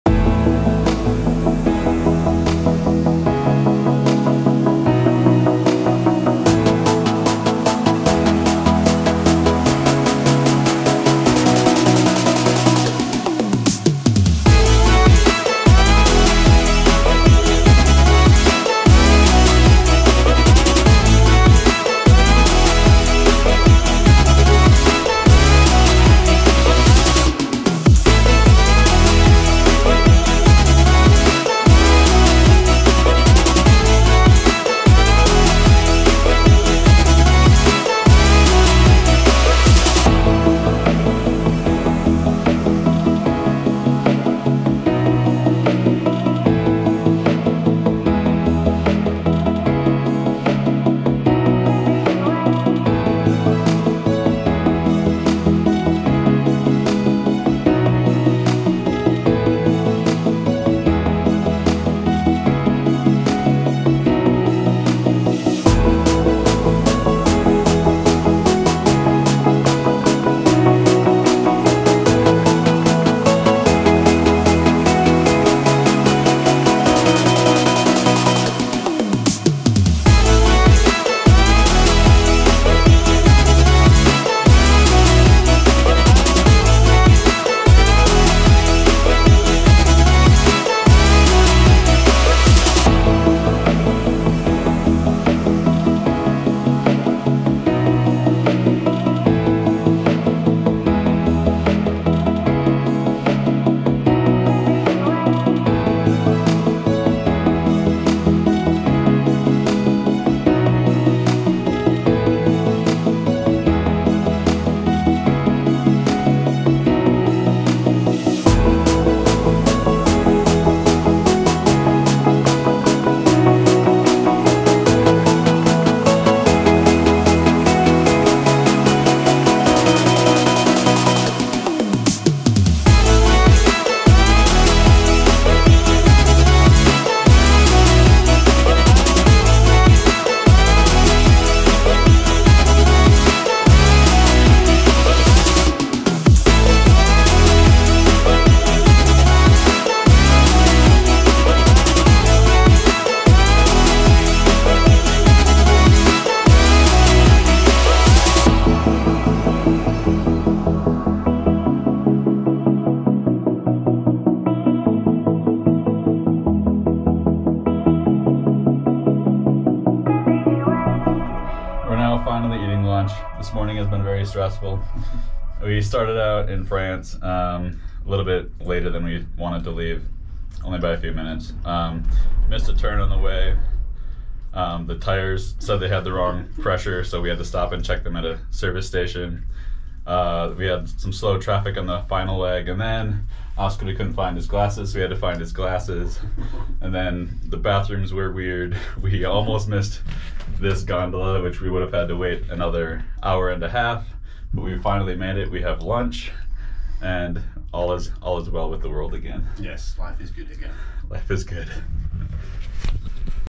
Climbing the Alps - Breithorn, Gran Paradiso, Vincent Pyramid - Around the World in 30 Days, Part 1
Part 1 of 3 in a series of videos highlighting a 30 day mission around the world in August 2022.

The first objective was to climb 3 glaciated 4,000 meter peaks in the European Alps. My friend Oskari and I traveled around northern Italy, France and Switzerland for 8 days while staying in a combination of alpine rifugios and bougie hotels. All three trips were a success with no injuries and summitting a peak over 4,000 meters.

Peak 1: Breithorn starting from Cervinia, staying 1 night at Refujio Guide del Cervino
Peak 2: Gran Paradiso, staying 1 night at Rifugio Chabod
Peak 3: Vincent Pyramid, staying two nights at Refugio Gnifetti.

The original plan for peak 3 was Mont Blanc but due to the extraordinarily warm temperatures that route was not safe. The backup plan was to attempt Zumsteinspitze, but due to whiteout conditions on the day we opted to go for Vincent Pyramid instead, which was a shorter route with fewer objective hazards.